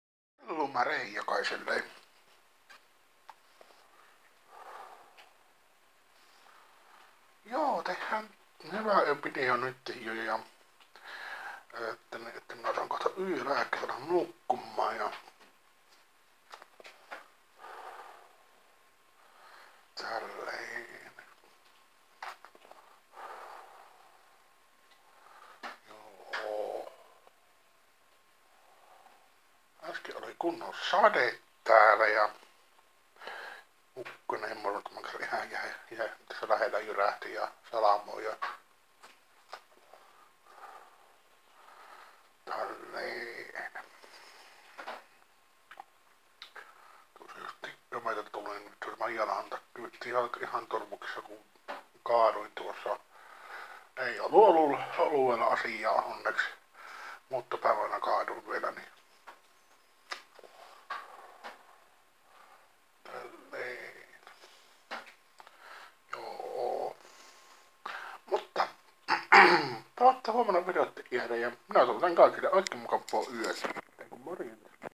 hyviä öitä
sataa ja ukostaa. .